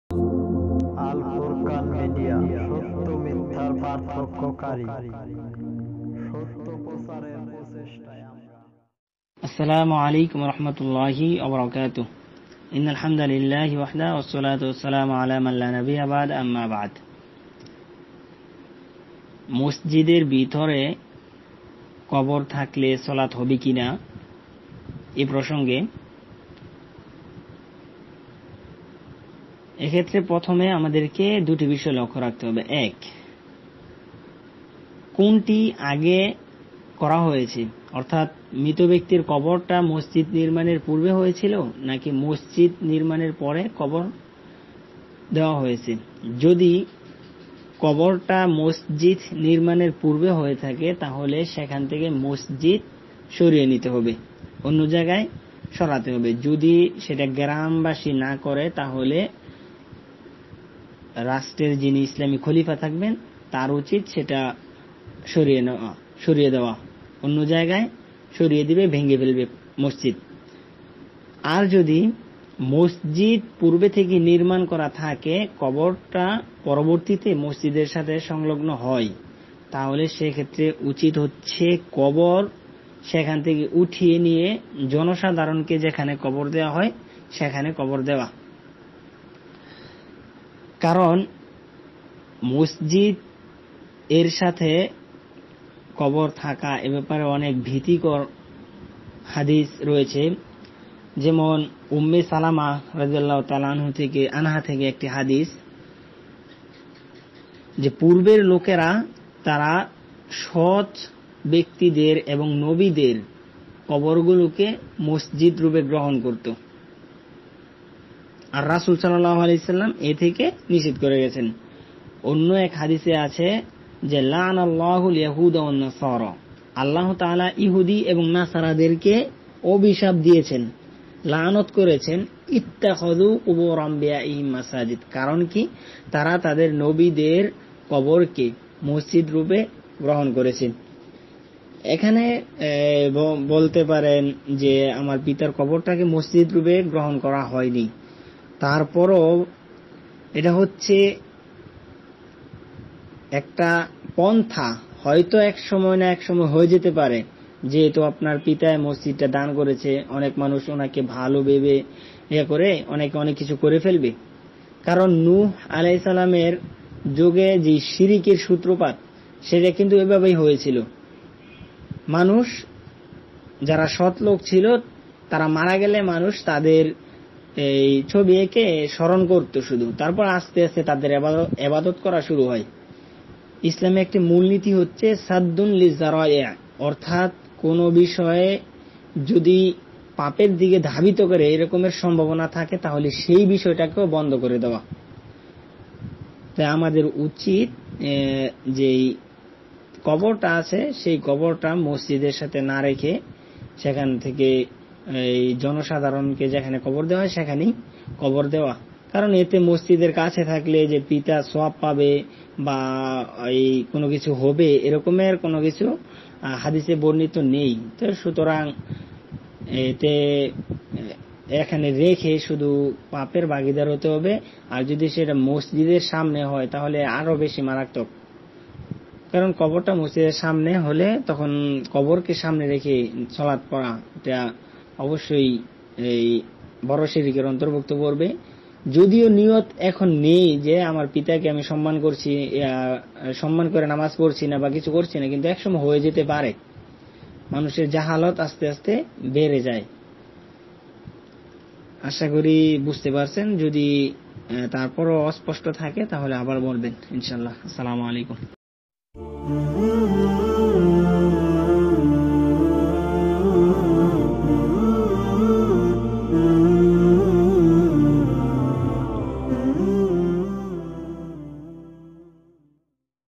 কবর কেন্দ্রিক মসজিদে নামাজ পড়া কি বৈধ?
মসজিদের ভিতরে কবর থাকলে সেখানে সালাত পড়ার বিধান কি?জোবায়ের বিন মজিবুর রহমান